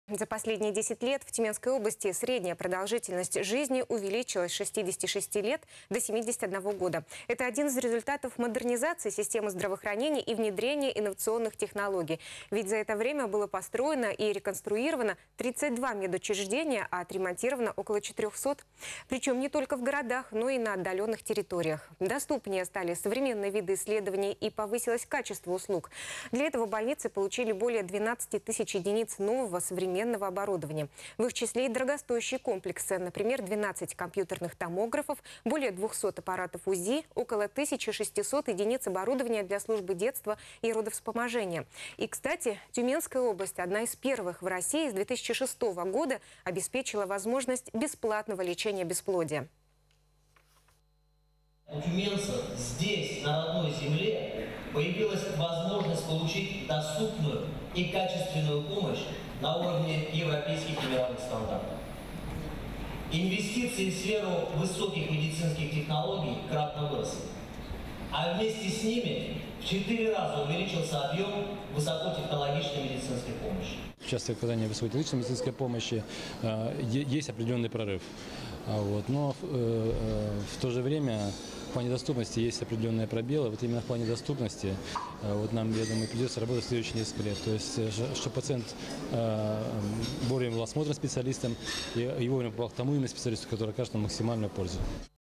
Модернизации системы здравоохранения помогла увеличить продолжительность жизни
За последние 10 лет в Тюменской области средняя продолжительность жизни увеличилась с 66 лет до 71 года. Это один из результатов модернизации системы здравоохранения и внедрения инновационных технологий. Ведь за это время было построено и реконструировано 32 медучреждения, а отремонтировано около 400. Причём не только в городах, но и на отдалённых территориях. Доступнее стали современные виды исследований, повысилось качество услуг. Для этого больницы получили более 12 тысяч единиц современного оборудования.